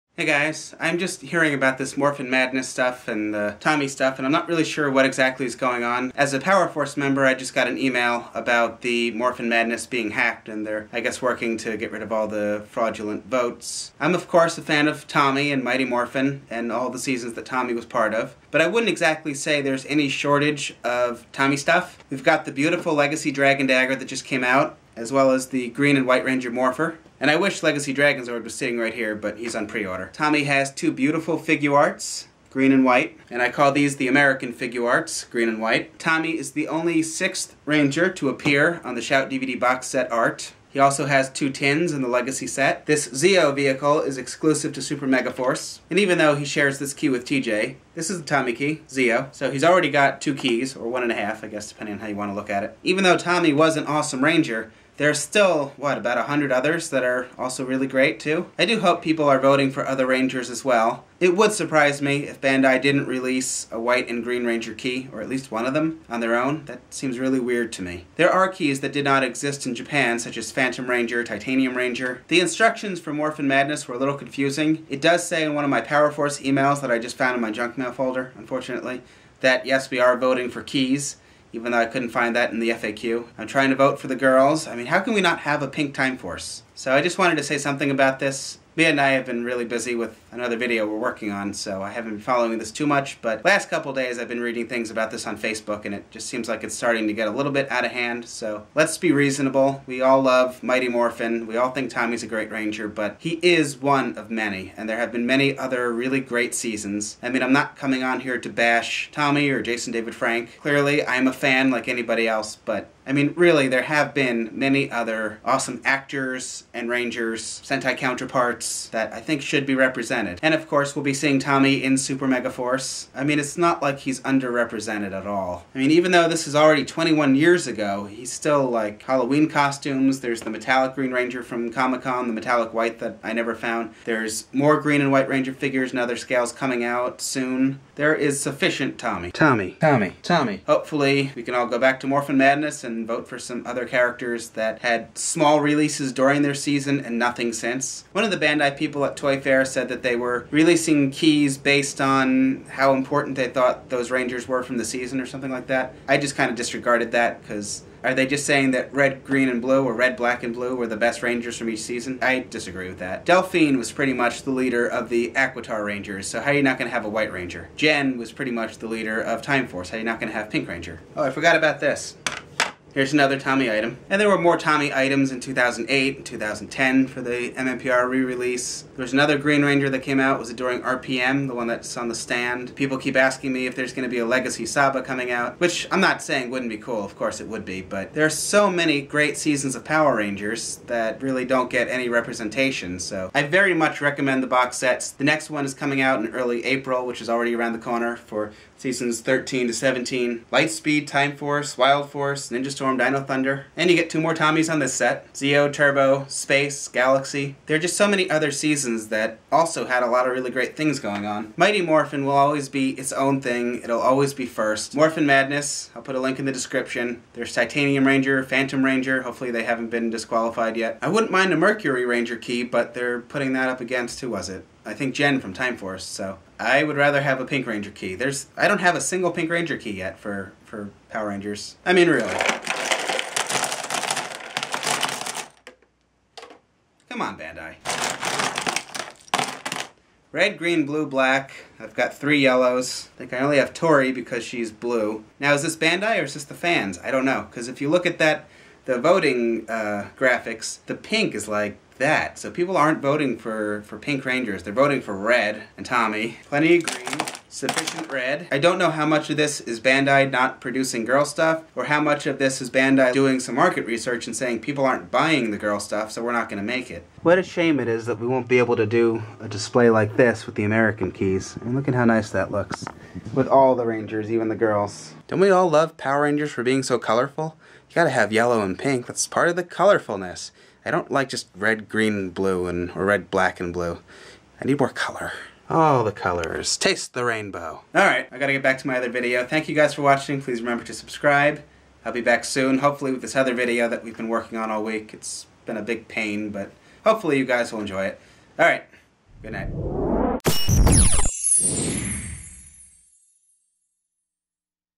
Morphin Madness Madness! (Power Rangers)
From Bandai:
Power Rangers fans, thank you for voting in Morphin Madness!

It appears some enthusiastic fans have managed to artificially inflate the vote tally in favor of certain Rangers.

In the interest of creating a fun and fair competition, we have carefully removed the extra votes and restored the rankings to reflect their true values.

We'll continue to closely monitor the results to ensure that Morphin Madness is decided fairly by the fans!

"Where do you buy toys?"

OUR MAILBOX:
Bruno/MMPRtoys
868 W. Street Rd. #345